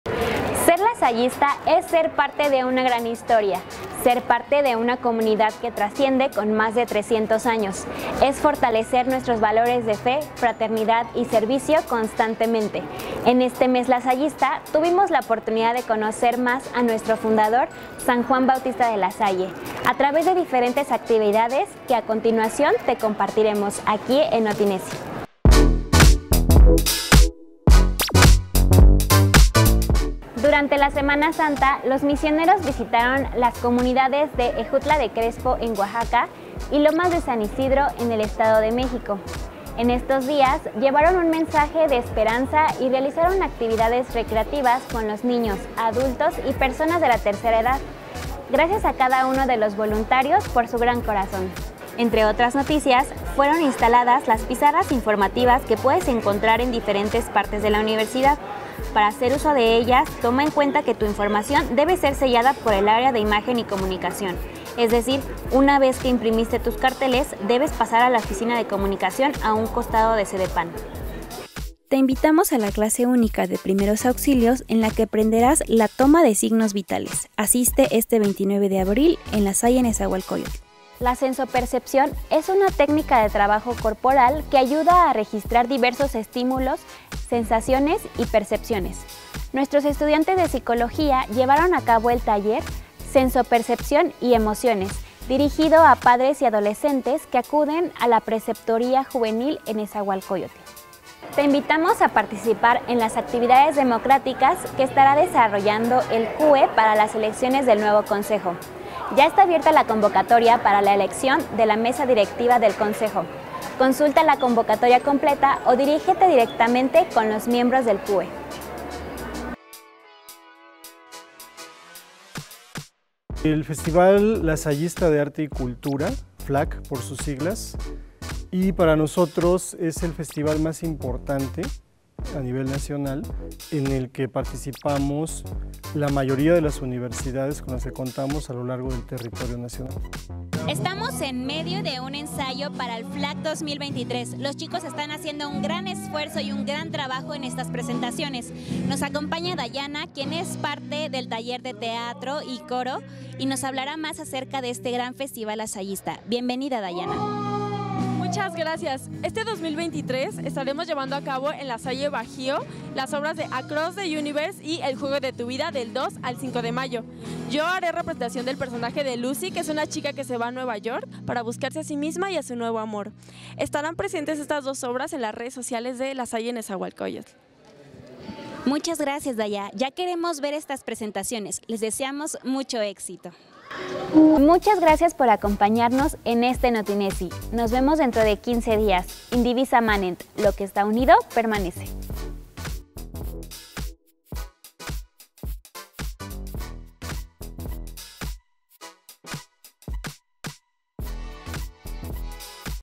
NotiNezzi 6
Querida comunidad, les compartimos la sexta edición de NotiNezzi en video.​
Entérate de las noticias de nuestra Comunidad:​

🔴 Experiencia con los msioneros.​
🔵 Nuevas Pizarras informativas.​
🔴 Curso de primeros auxilios.​
🔵 Práctica en la preceptoría.​
🔴 Participación en la Comisión Electoral Estudiantil.​
🔵 Festival Lasallista de Arte y Cultura 2023.​

¡No te pierdas el próximo NotiNezzi!​